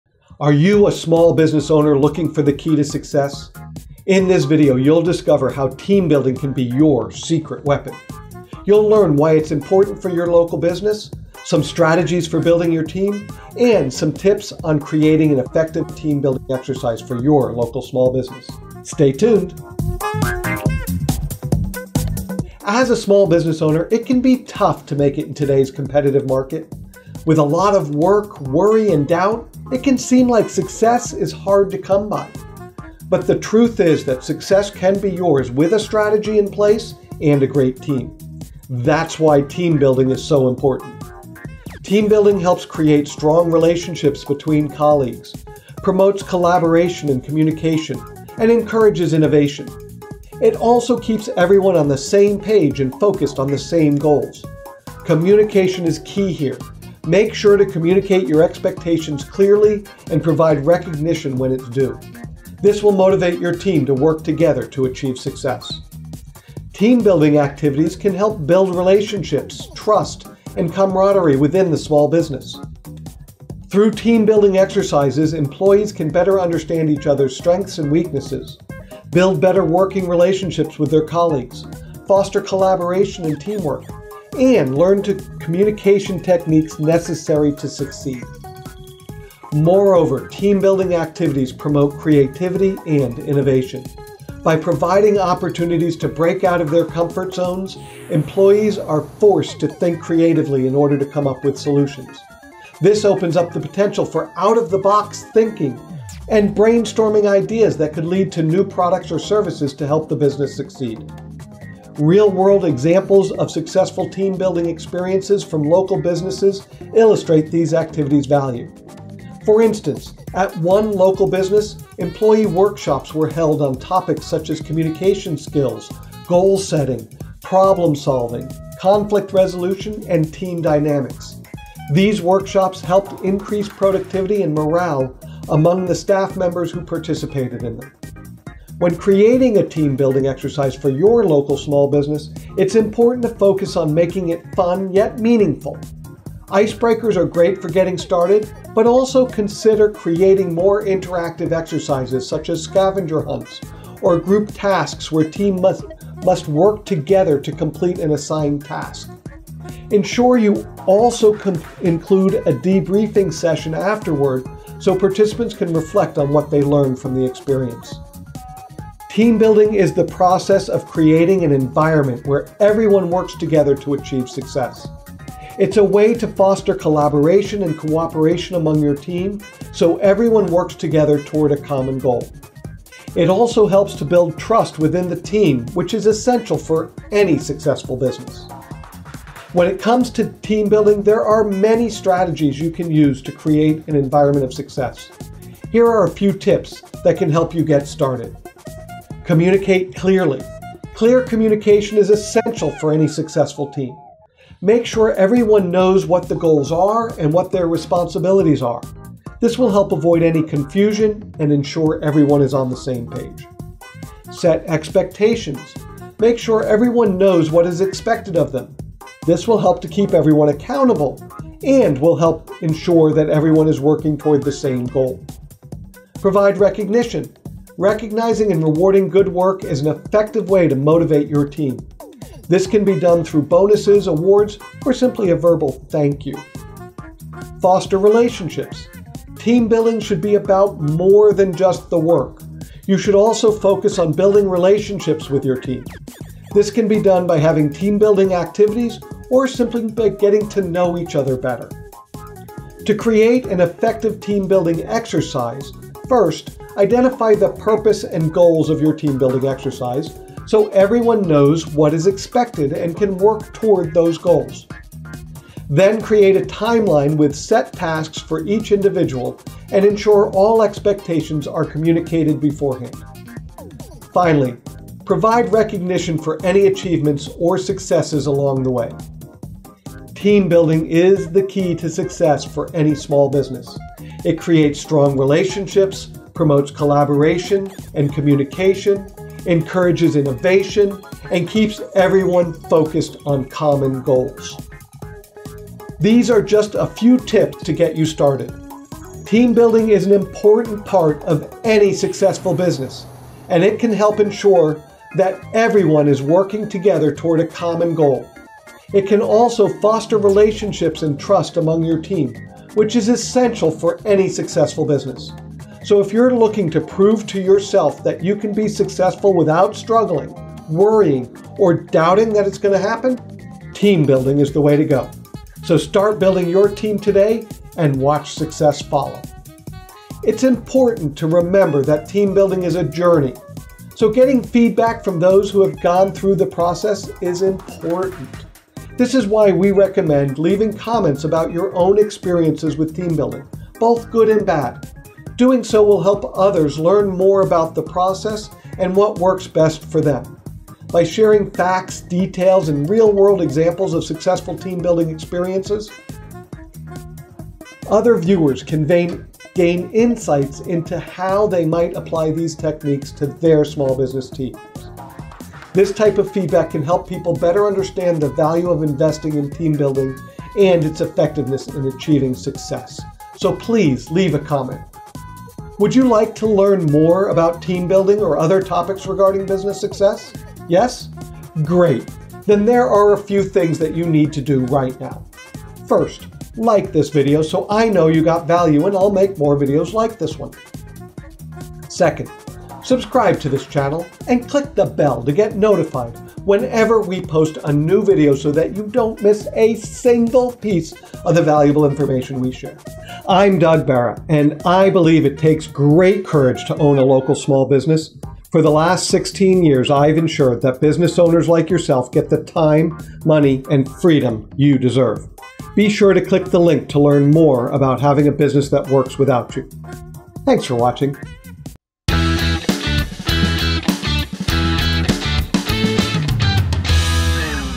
Unlock the Key to Achieving Success with Team Building - Business Success - Miami, FL
Would you like to have a business that works without you?

Are you a small business owner looking for the key to success?

In this video, you'll discover how team building can be your secret weapon.

You'll learn why it's important for your local business, some strategies for building your team, and some tips on creating an effective team-building exercise for your local small business.